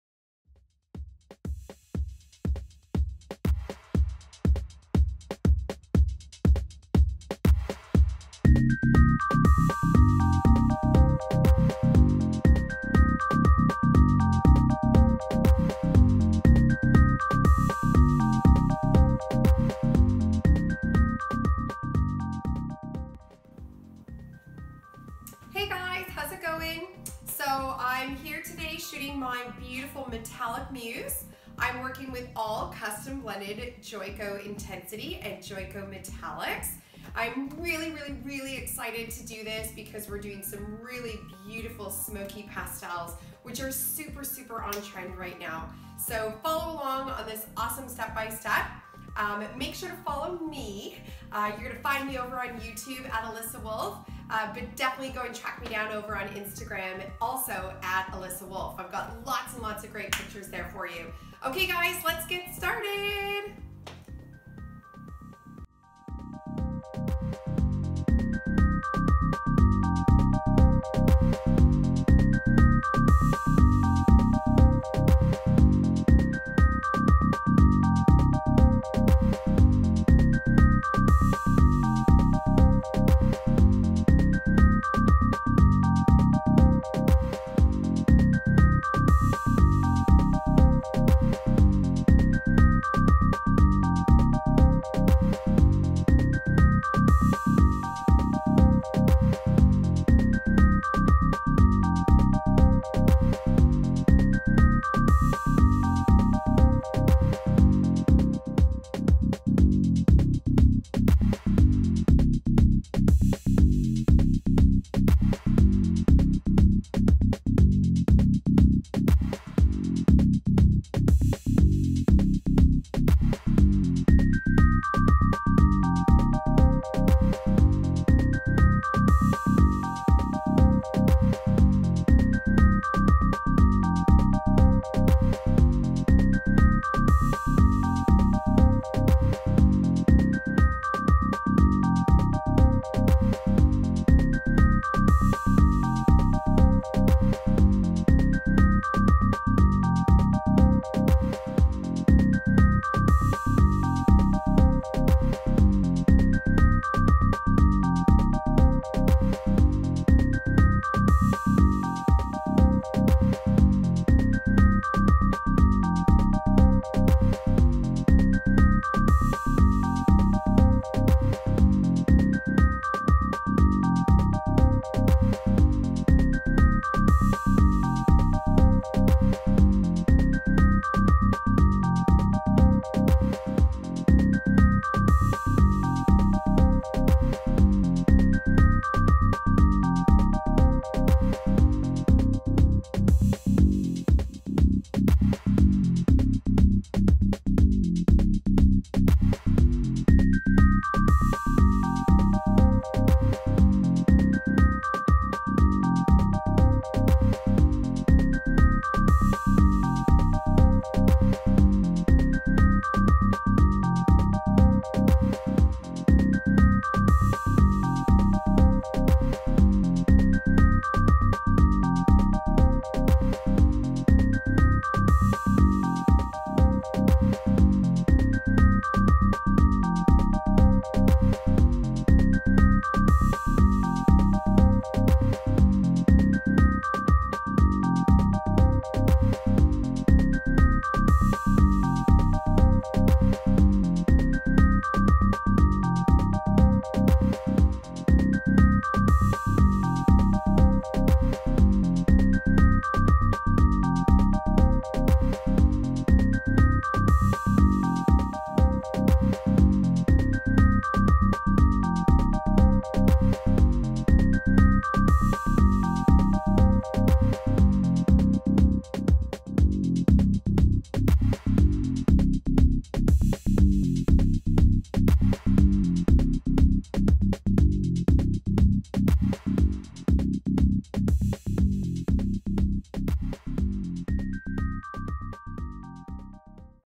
Metallic Melted Pastel Lob
In this video,I am going to show you behind the scenes and how-to techniques to my metallic muse photo entry for joico intensity.I started with a pre lightened and toned base of a level 10.I custom blended all Joico intensity shades with their new metallics for a smokey finish.I wanted to mix cool and warm tones together in a diagonal,chevron sectioned application.Melting colors by blending similar tones, and pops of warmth for high contrast.Make sure to go to my IG page for finished picture and more ! Enjoy!